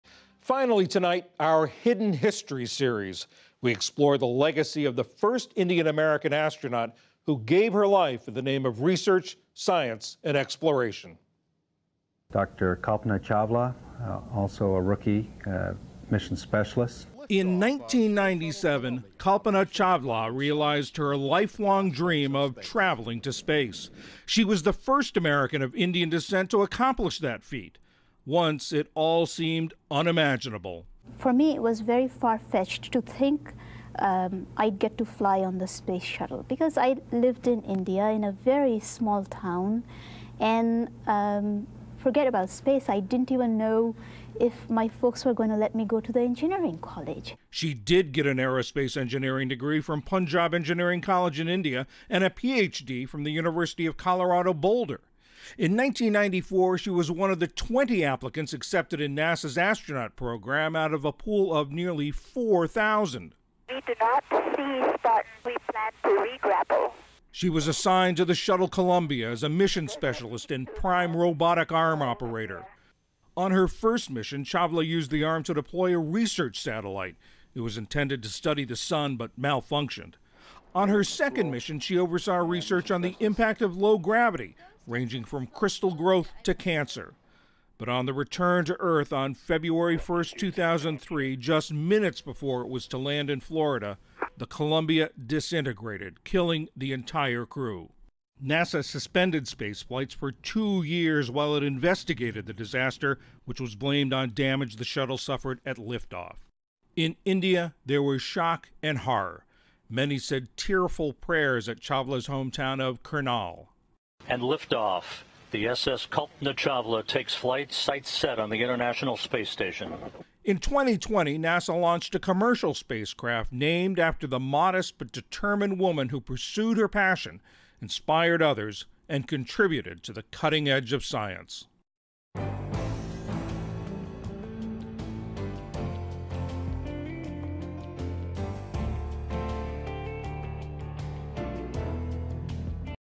Remembering Kalpana Chawla, the first Indian American to go to space
This Asian American and Pacific Islander Heritage Month, we’re highlighting people whose contributions have often been overlooked. Tonight, we explore the legacy of Kalpana Chawla, the first American of Indian descent to travel to space, who gave her life in the pursuit of research, science and exploration.